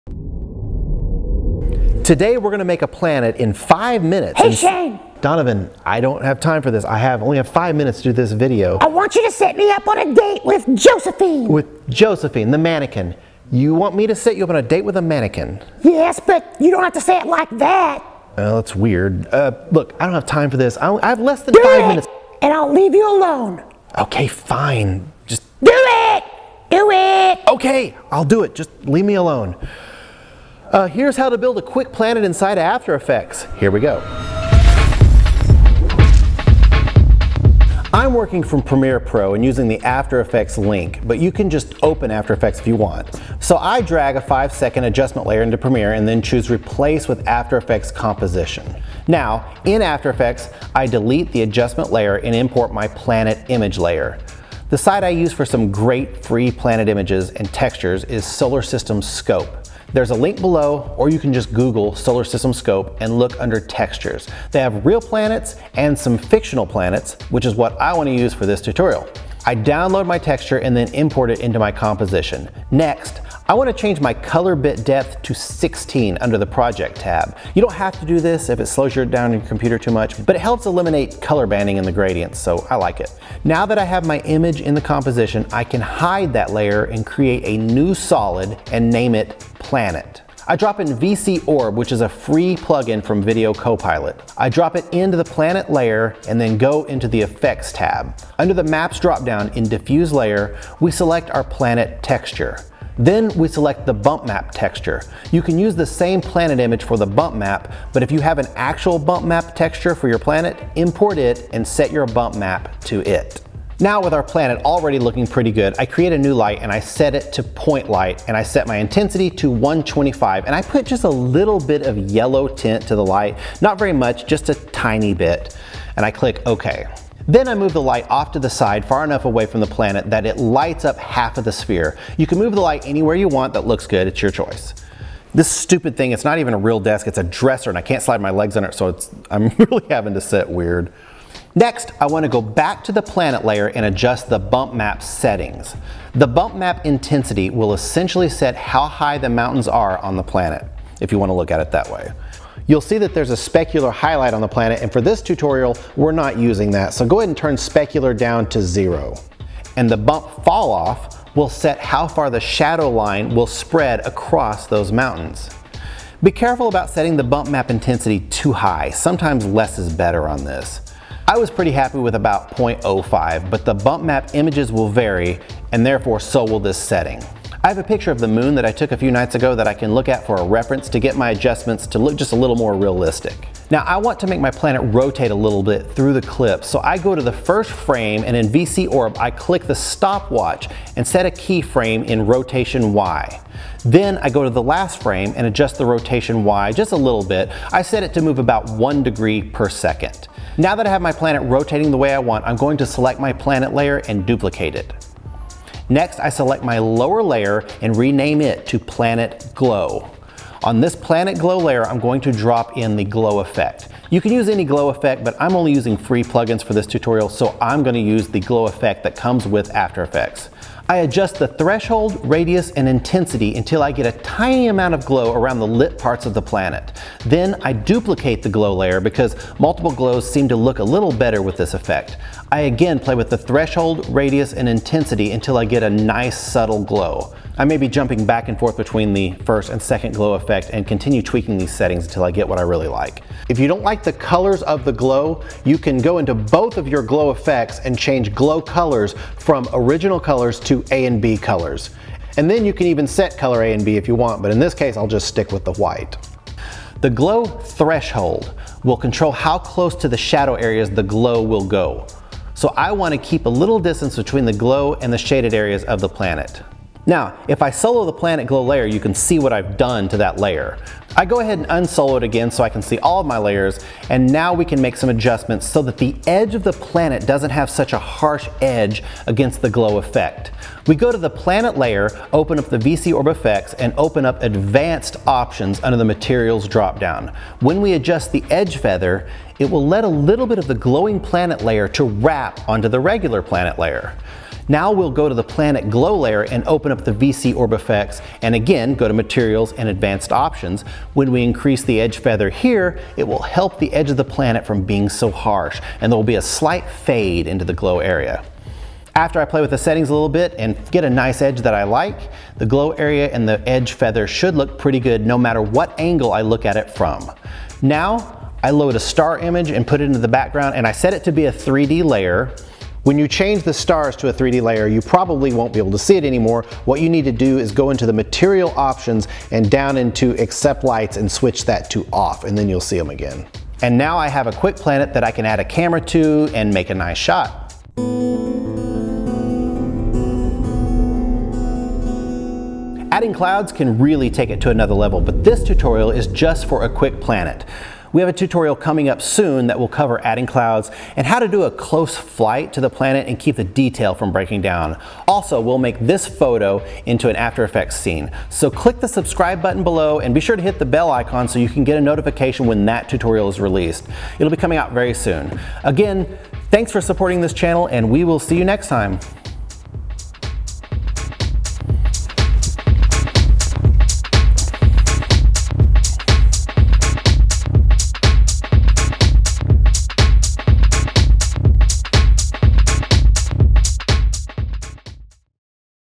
How to create a PLANET in 5 minutes with VIDEO COPILOT ORB - After FX
In this video, we show a quick way to get a great looking planet for After Effects.

Video Copilot Orb - Free VC Orb plugin for After Effects (Awesomeness!)-

How to create a PLANET in 5 minutes with VIDEO COPILOT ORB - After Effects Tutorial